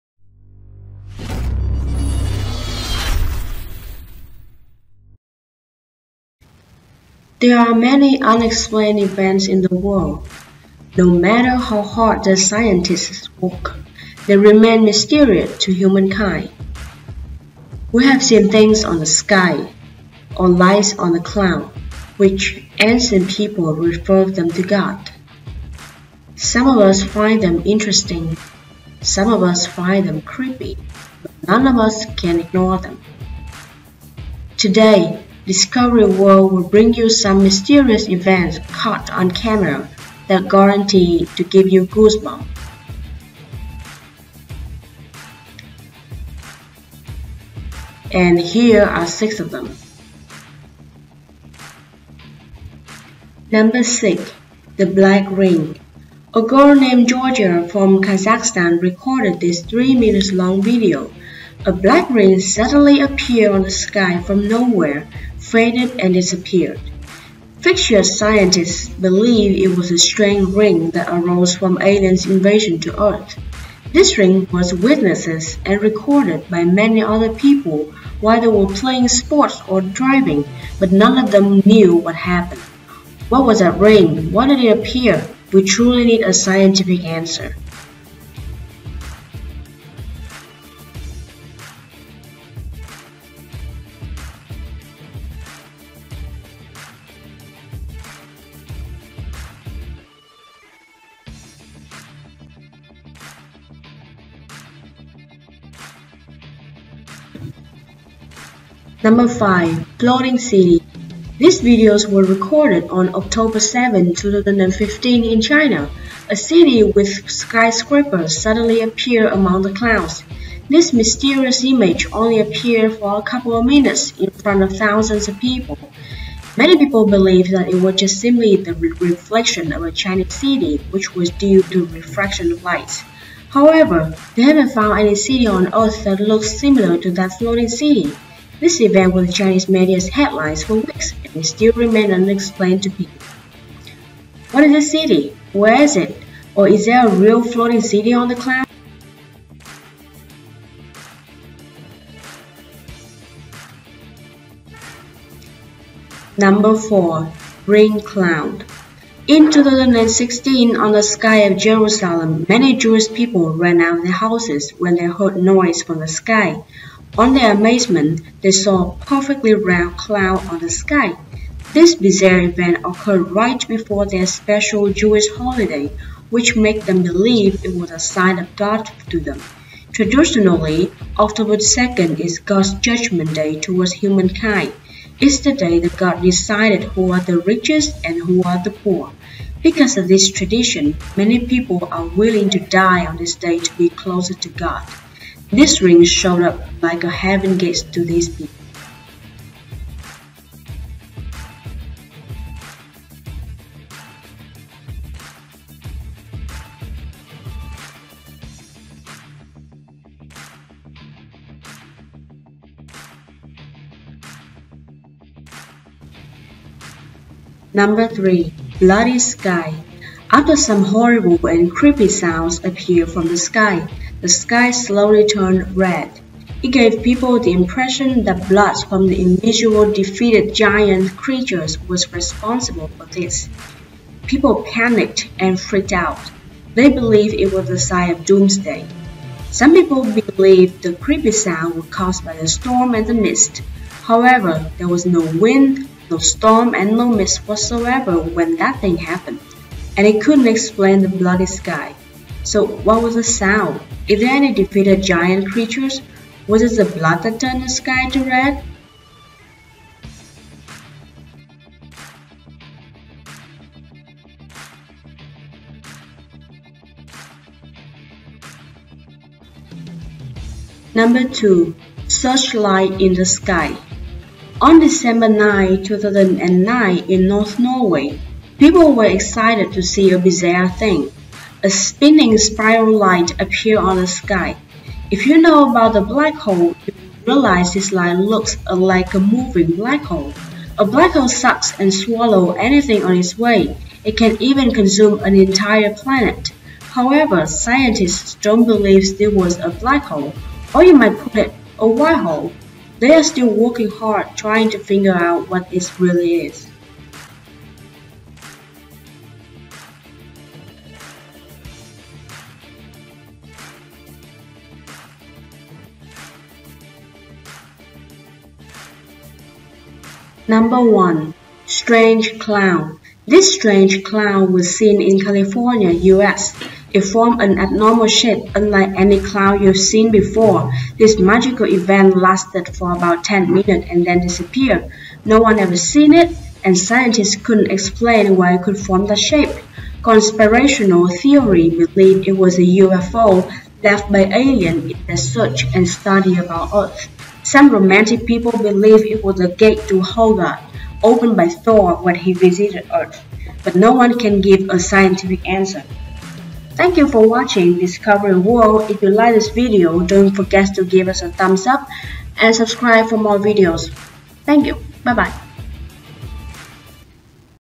Discovery World | 6 mysterious events caught on camera that scientists are working hard to solve
Welcome to Discovery World!
There are many unexplained events in the world. No matter how hard the scientists work, they remain mysterious to human kind. We have seen things on the sky or lights on the cloud which ancient people refered them to God. Some of us find them interesting, some find them creepy but none of us can ignore them. Today Discovery World will bring you some mysterious events caught on camera that guarantee to give you goosebumps.
Here are 6 of them.
6 mysterious events caught on camera that scientists are working hard to solve